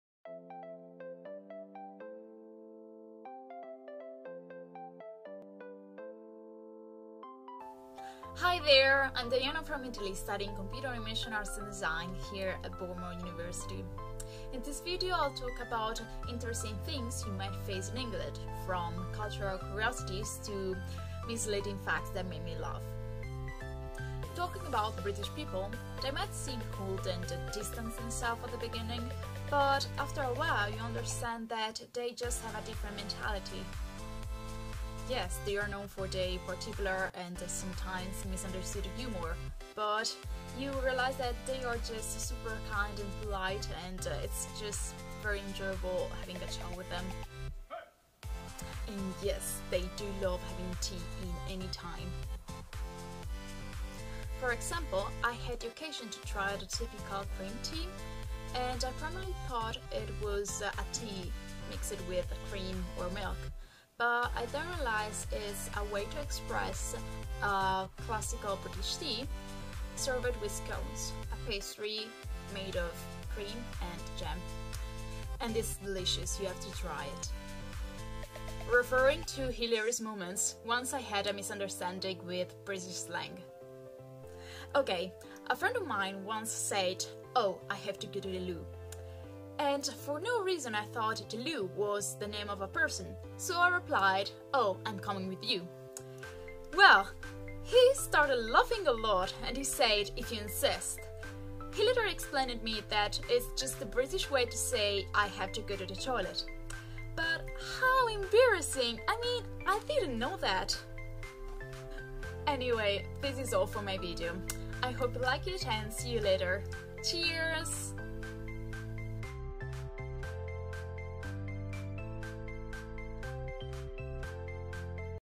British culture through the eyes of an international student
Diana Pelino is from Italy and is studying BA (Hons) Computer Animation Arts and Design. In this vlog she tells us some fun facts about the British culture: the people, the tea and the English slang!

Diana Pelino, Italy, BA (Hons) Computer Animation Arts and Design, 2017/18.